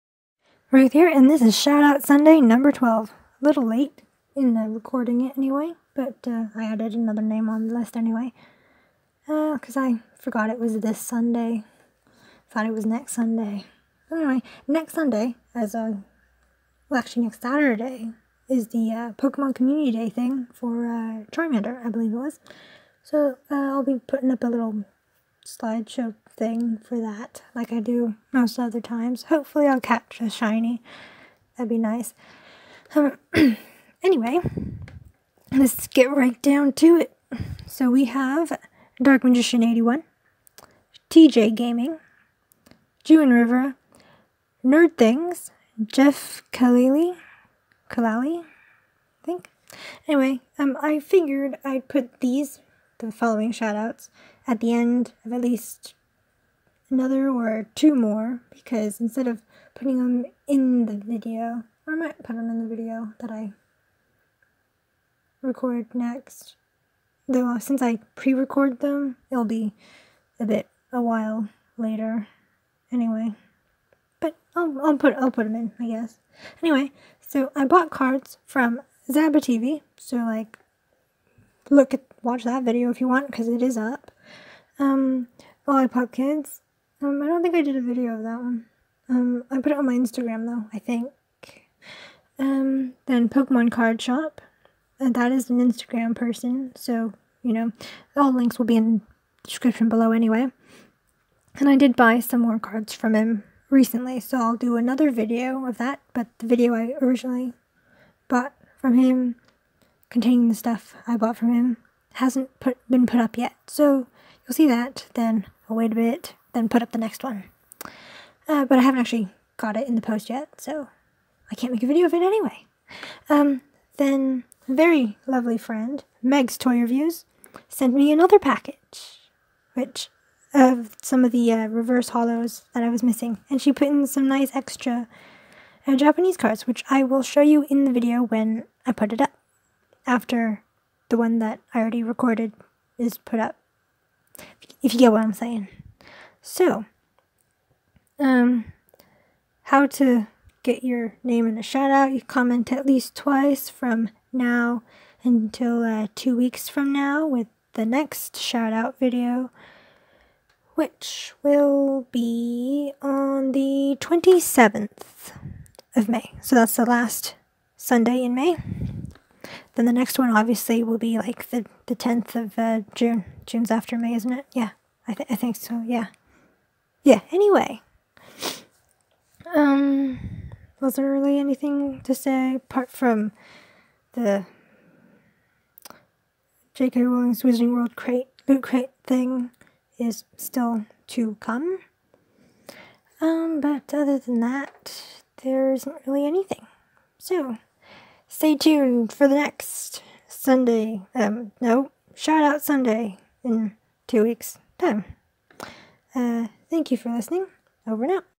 Shout Out Sunday #12
People that have commented at least twice from my last shout out video!

And I will try to get to all or most in the next video!

Another way to get a Shout Out it comment at least 2 times from now to the next Shout Out Video! ^.^

I can Shout Out a youtube channel or other  social media like … fb, twitter, instagram, snupps, etc…
Send link to that page in email below